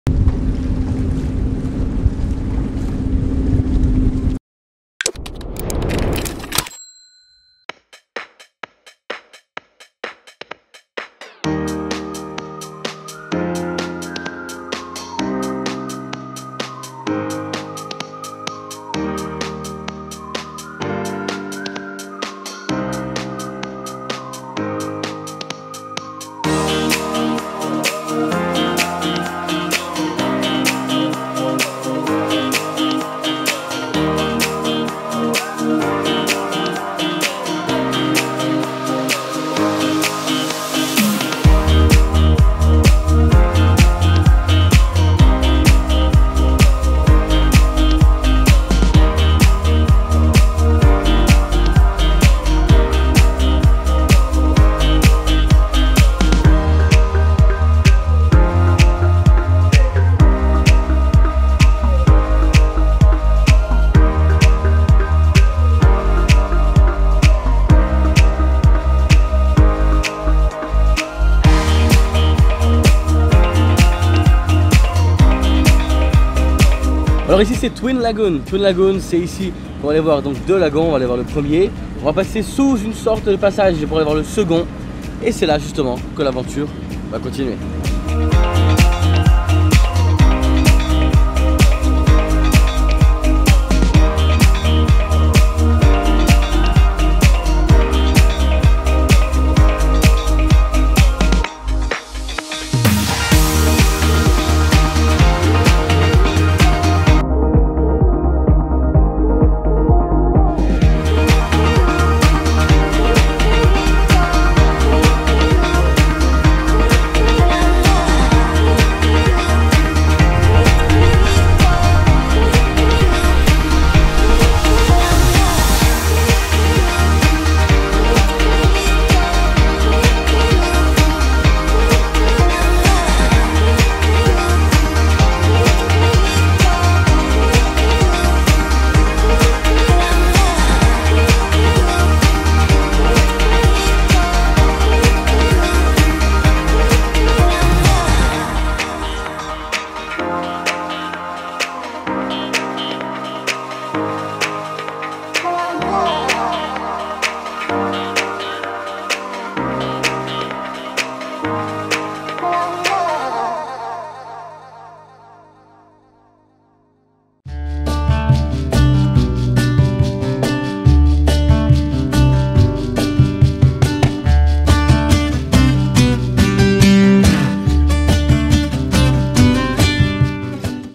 Philippines - Visiter les îles et plages secrètes (Pas si secrète)
C'est parti, on se rend de nouveau aux Philippines et plus précisément à Coron, dans les îles ou je vous embarque dans un bateau local à travers les eaux turquoises... Incroyable !